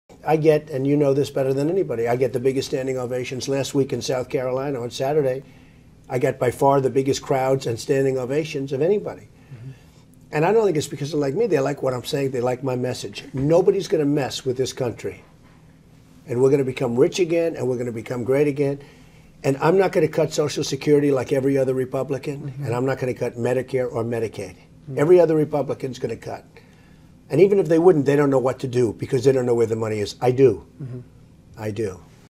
Donald Trump vs.Other Republicans
Donald Trump tells The Brody File he will not cut Medicaid, Medicare or Social Security, should he run for president in 2016.